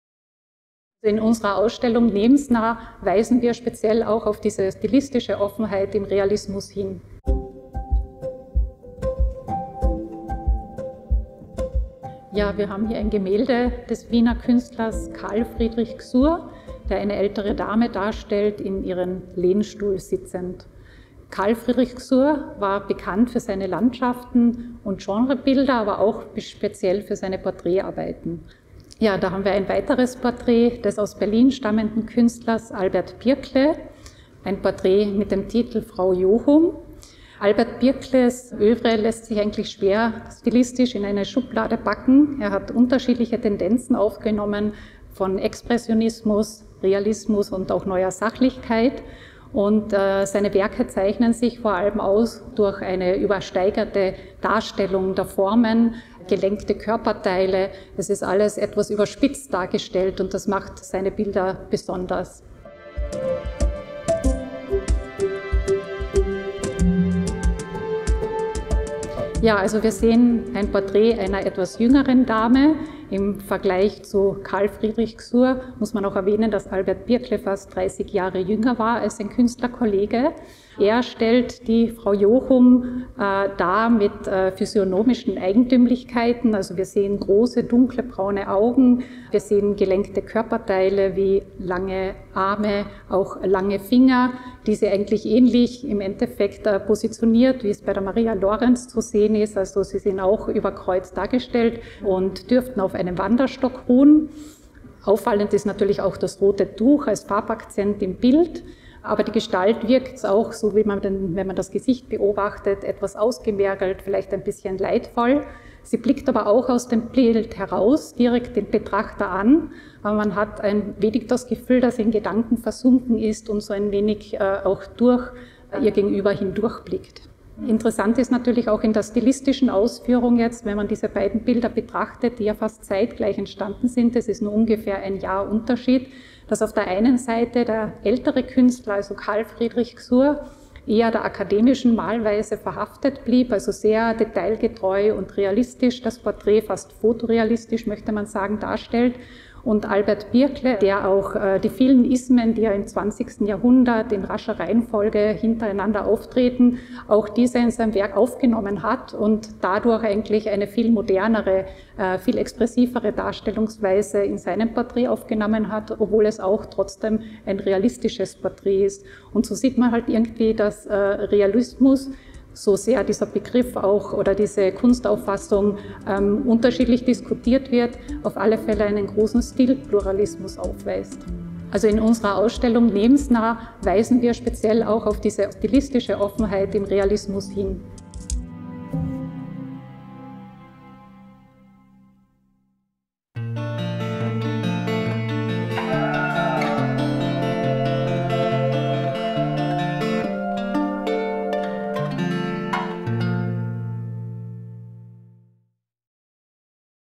Lebensnah. Realistische Malerei von 1850 bis 1950 (4/4)
Lebensnah
Realistische Malerei von 1850 bis 1950

18. März 2022 - 1. November 2022 im Oberes Belvedere

Ist alles realistisch, was lebensnah scheint? Was fasziniert Betrachter*innen an der detailgetreuen Wiedergabe der Realität? Jedes Abbild der Wirklichkeit kann in der bildenden Kunst generell als realistisch bezeichnet werden. Dennoch: Durch die Fokussierung auf bestimmte Inhalte will realistische Malerei – im Gegensatz zum Naturalismus, dessen Maßstab die äußere Richtigkeit ist – tiefer liegende Wahrheiten vermitteln. Die neue Sonderschau im Oberen Belvedere präsentiert mit Lebensnah realistische Werke aus einem Zeitraum von einhundert Jahren.

Kuartiert von Kerstin Jesse und Franz Smola.